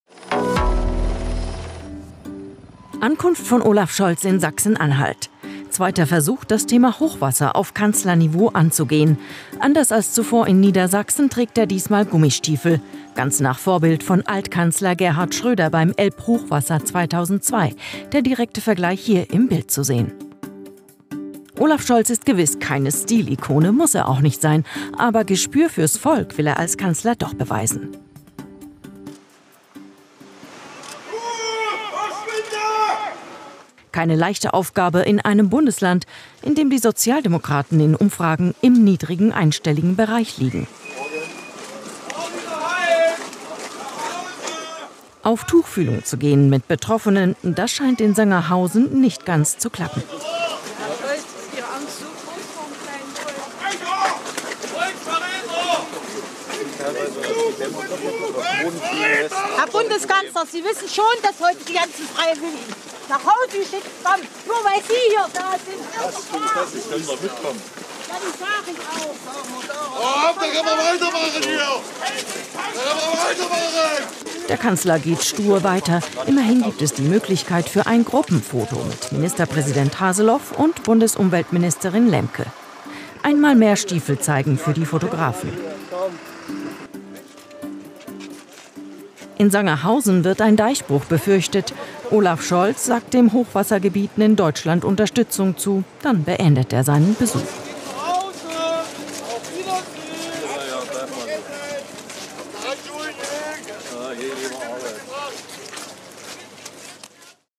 Hochwasser: Olaf Scholz bei Besuch in Sachsen-Anhalt bepöbelt | DER SPIEGEL
Diesmal hatte Olaf Scholz wasserfestes Schuhwerk dabei: Mit seinem zweiten Besuch in den Flutgebieten wollte der Kanzler politisch punkten. In Sachsen-Anhalt erwarteten ihn allerdings Regen und wütende Bürger.